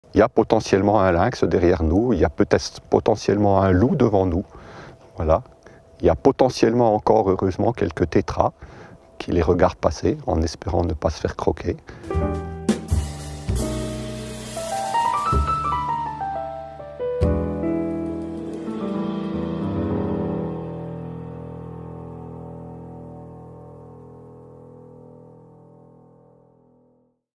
#11 "Il y a peut-être un lynx derrière nous" ?
Soirée participative du jeudi 9 décembre, organisée par le CAUE du Jura
Quel cadre de vie en territoire rural ?
Quels nouveaux enjeux citoyens ?
18 H 00 - Salle du Boeuf sur le Toit - Lons le Saunier
Soirée ouverte à toutes et à tous,